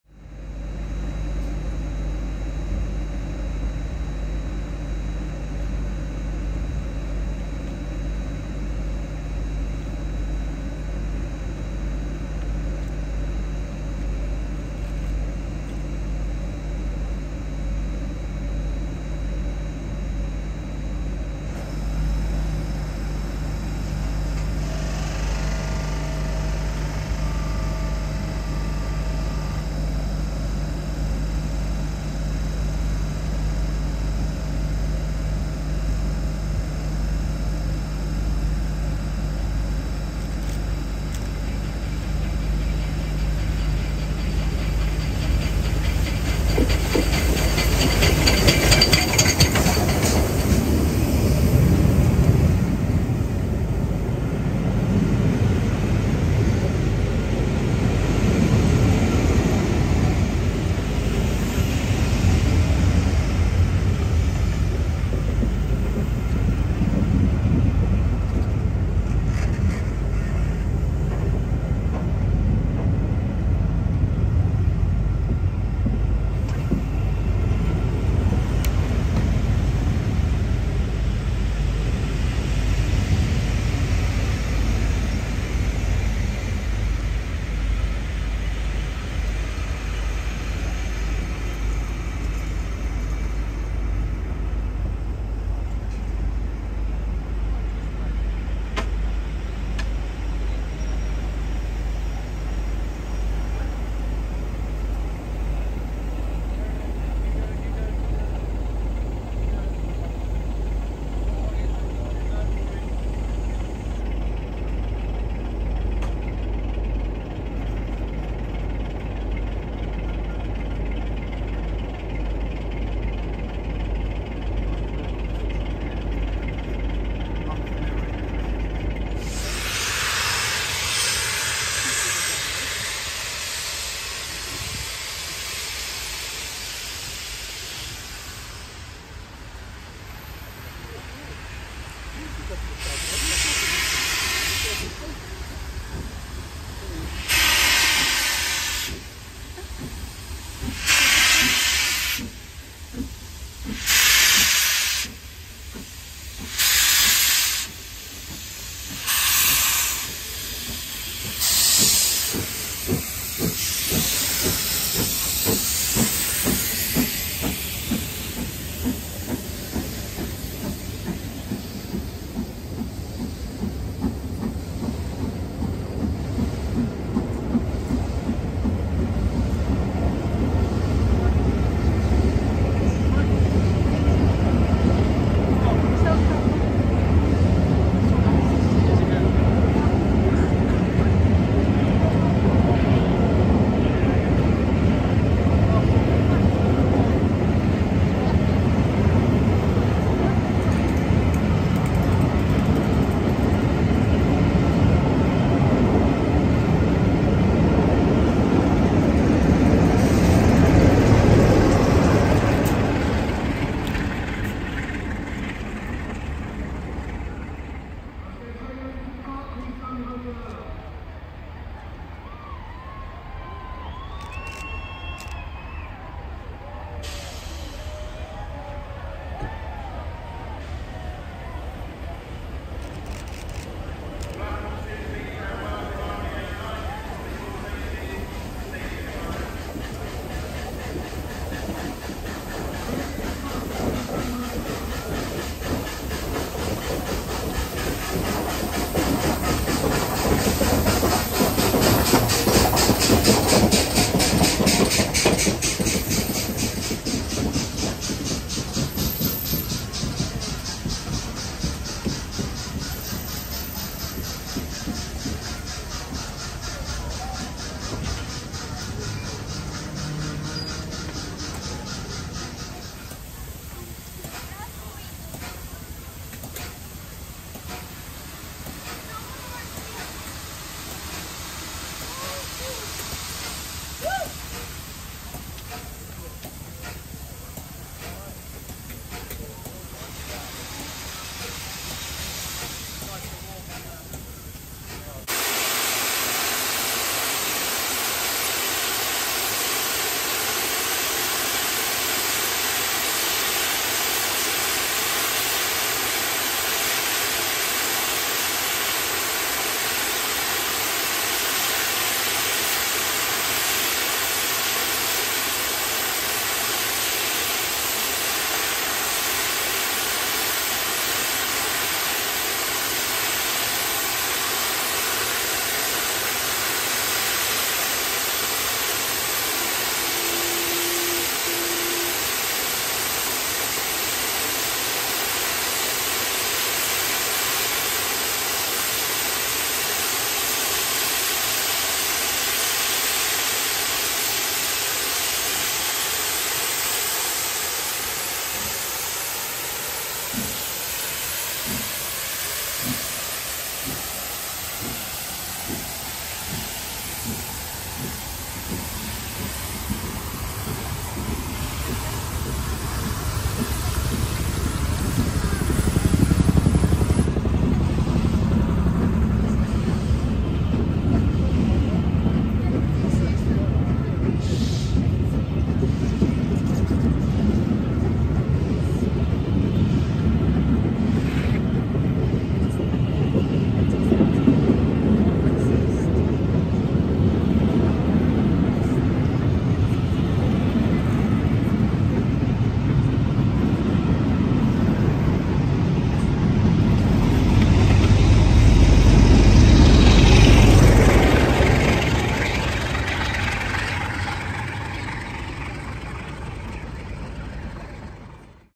Duchess of Sutherland 6233 bath spa
Duchess of sutherland at bath spa on a festively frosty day
The final steam tour of 2022 to bath and Bristol